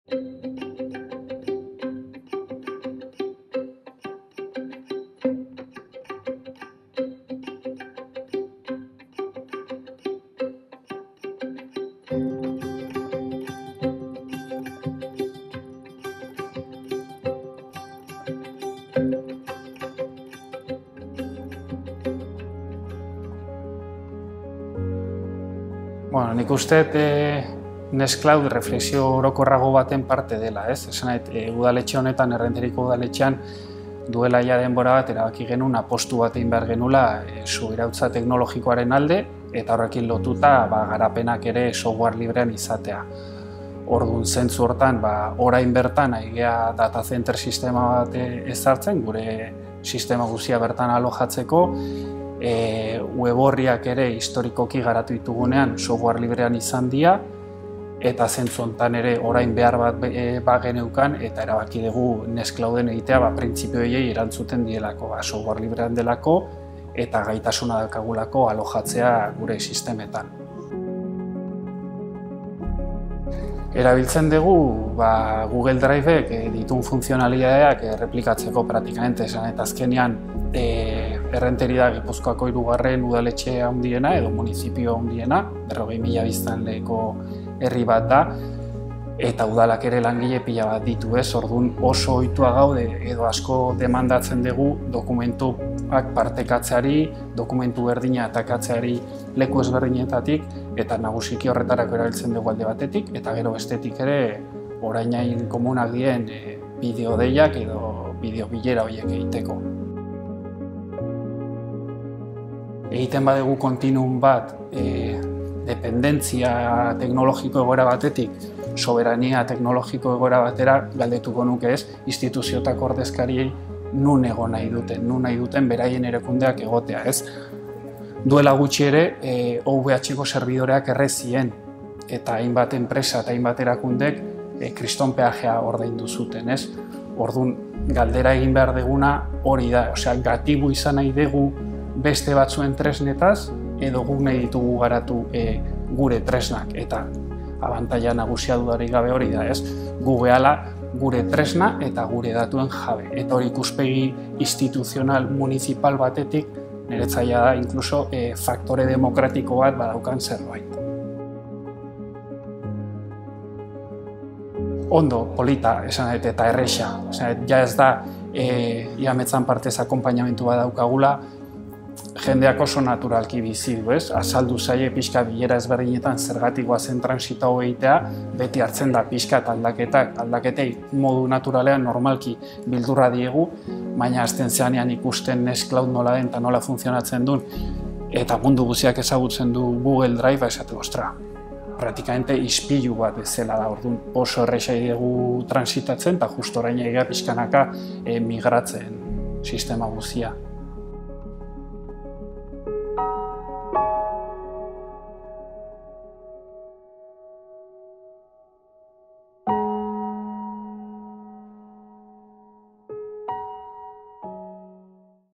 Nextcloud - Errenteriako udala
Nextcloud testigantzak jaso ditugu honetan. Hirugarrena Errenteriako udala.